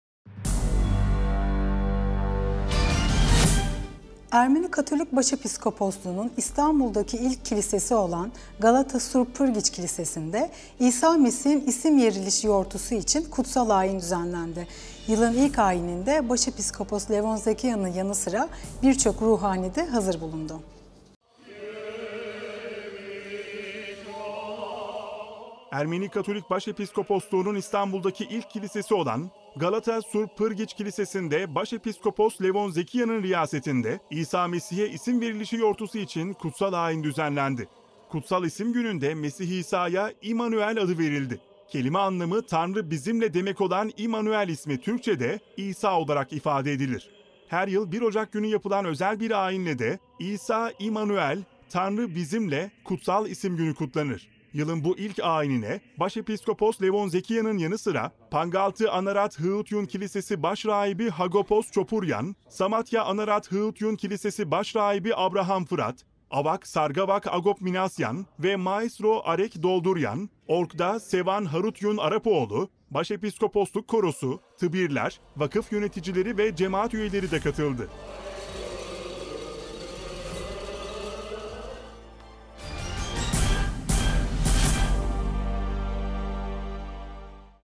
Ermeni Katolik Kiliseleri’nde İsa Mesih’e İsim Verilişi Yortusu | 10.01.2020
Ermeni Katolik Başepiskopluğu’nun İstanbul’daki ilk kilisesi olan;  Galata Surp Pırgiç Kilisesi’nde Başepiskopos Levon Zekiyan’ın riyasetinde, İsa Mesih’e İsim Verilişi Yortusu için kutsal ayin düzenlendi.

Kutsal  İsim Günü’nde Mesih İsa’ya “İmmanuel”  adı verildi. Kelime anlamı “Tanrı bizimle” demek olan İmmanuel  ismi,  Türkçe’de  “İsa” olarak ifade edilir. Her yıl  1 Ocak günü yapılan özel  bir ayinle de, İsa-İmmanuel (Tanrı bizimle) Kutsal İsim Günü  kutlanır.
Yılın bu ilk ayinine; Başepiskopos Levon Zekiyan’ın yanı sıra, Pangaltı Anarat Hığutyun Kilisesi Başrahibi Hagopos  Çopuryan, Samatya Anarat Hığutyun Kilisesi Baş Rahibi Abraham Fırat, Avak Sargavag Agop Minasyan  ve  Maestro Arek Dolduryan , org’da Sevan Harutyun Arapoğlu,  Başepiskoposluk  korosu, tıbirler, vakıf yöneticileri  ve cemaat üyeleri  de katıldı.